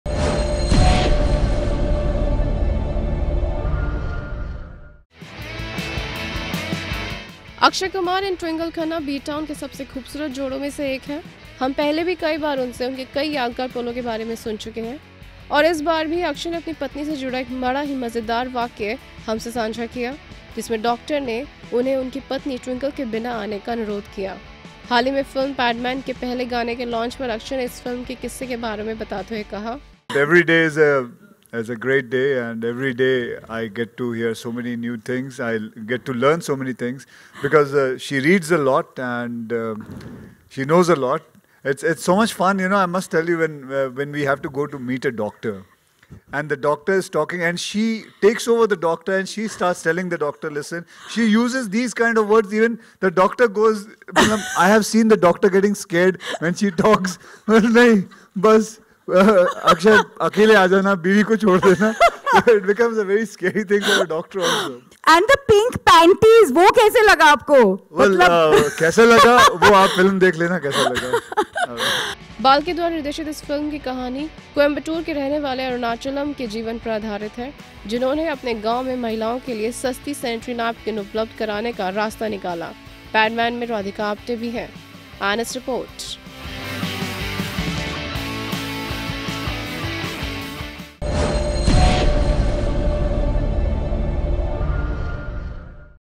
डॉक्टरों ने क्यों Akshay को Twinkle के बिना आने का अनुरोध किया ?
अक्षय कुमार और ट्विंकल खन्ना बी-टाउन के सबसे  खूबसूरत जोडों में से एक हैं । हमने पहले भी कई बार उनसे उनके कई यादगार पलो के बारे में सुन चुकें हैं और इस बार भी अक्षय ने अपने पत्नी से जुडा एक बडा ही मजेदार वाक्या हमसे साझा किया  , जिसमें डॉक्टरों ने उन्हें उनकी पत्नी ट्विंकल के बिना आने का अनुरोध किया ।